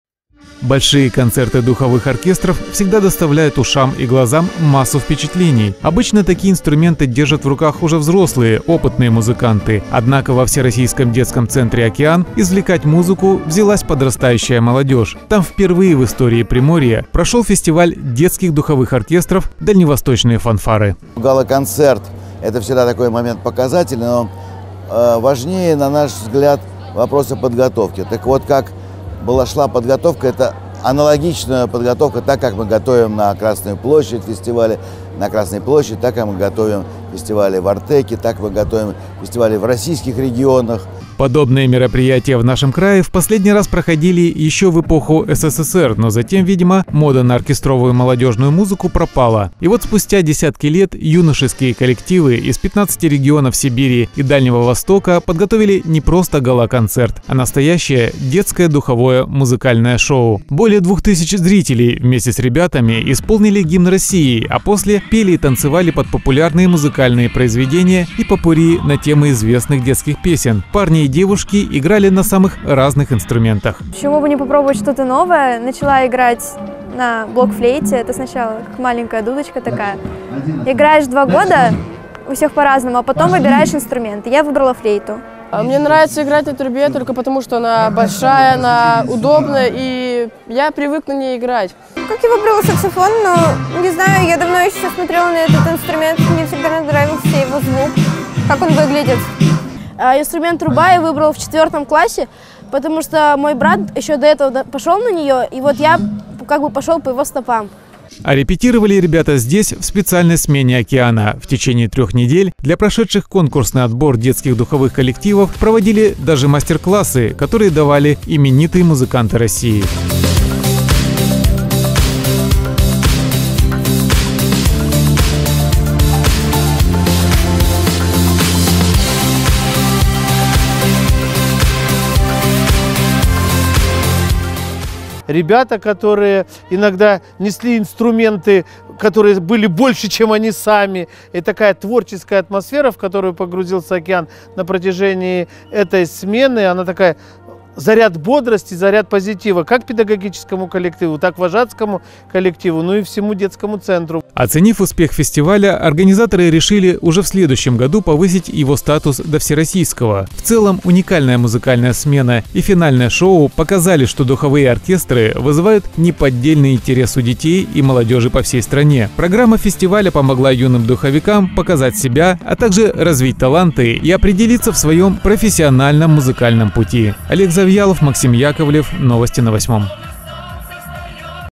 «Дальневосточные фанфары»: детская оркестровая музыка во Владивостоке
Юношеский гала-концерт «Дальневосточные фанфары» прошел во Владивостоке. Оркестровое шоу стало итогом целой смены, которую принимал всероссийский детский центр "Океан". Туда съехались 15 коллективов Дальнего Востока и Сибири, которые провели 3 музыкальные недели, насыщенные мастер-классами и подготовкой к итоговому концерту.